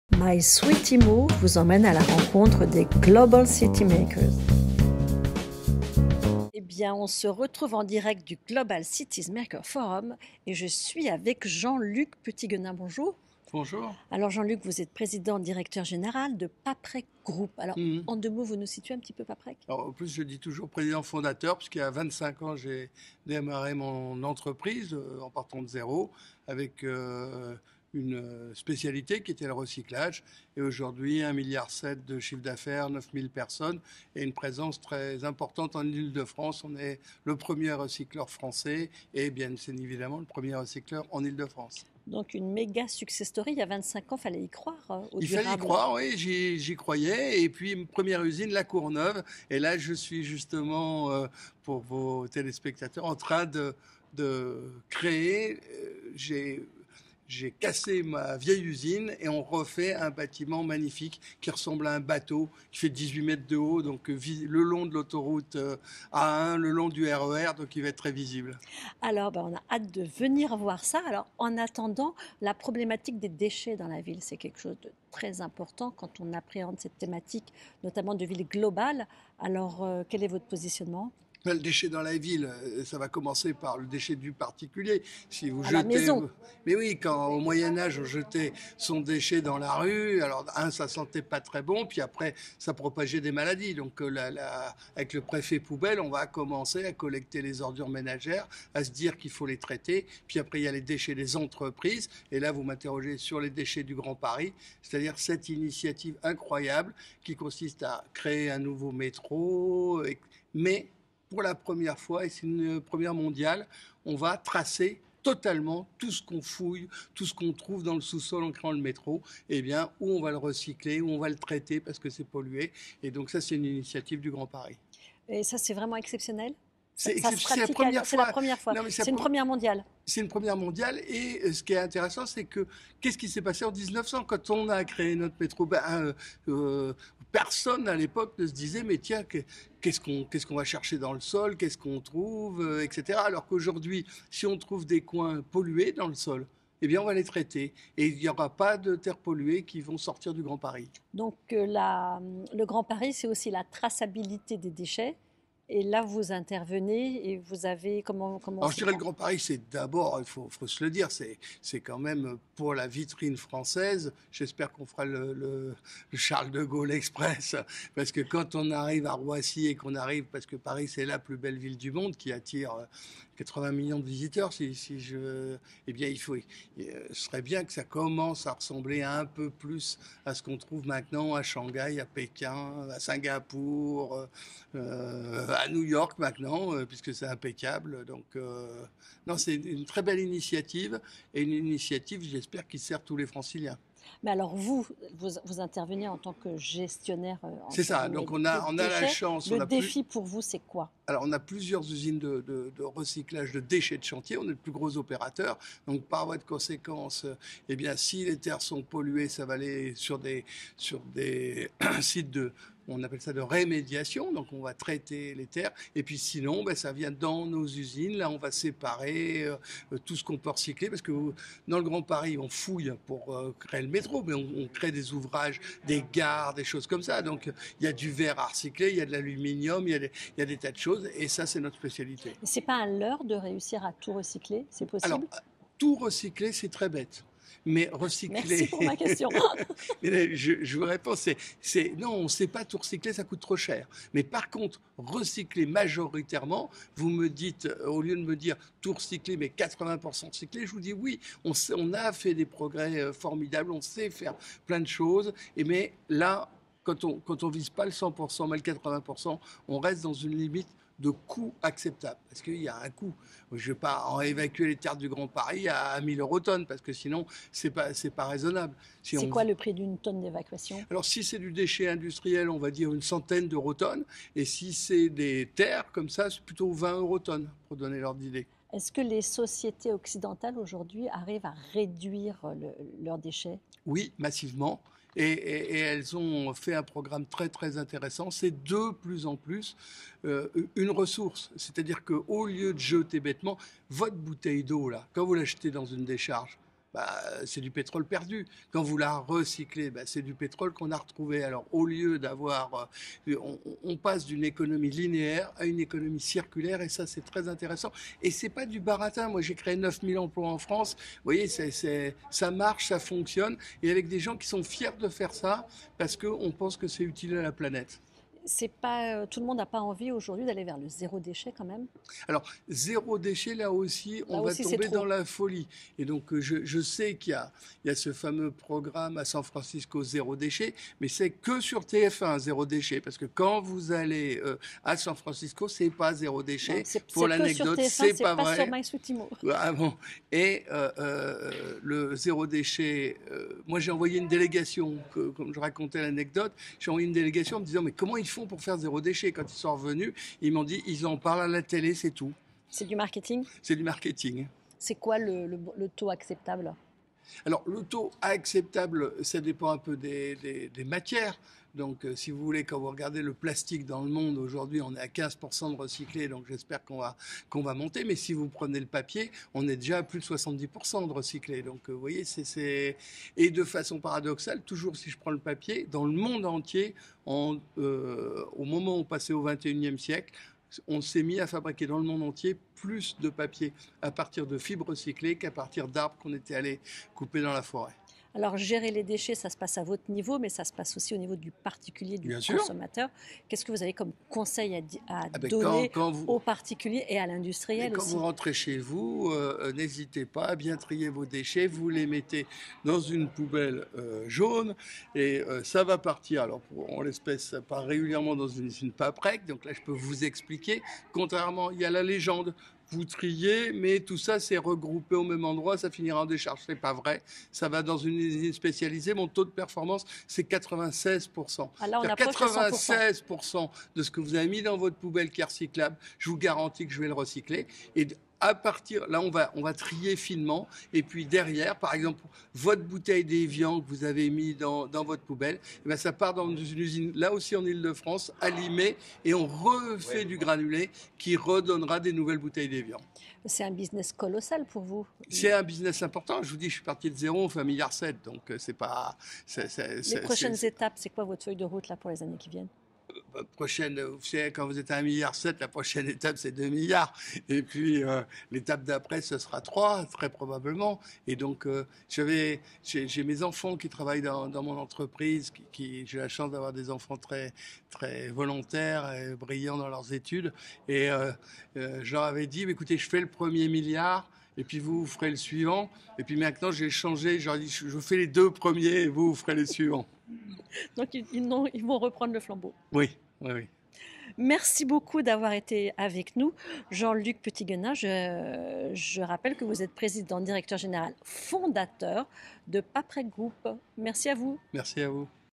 Jean-Luc Petithuguenin PDG de PAPREC au forum
Retrouvez Jean-Luc Petithuguenin, président-directeur général de PAPREC au micro d' Ariane Artinian à l'occasion du premier forum Global Cities Makers, organisé par Paris Capitale Economique.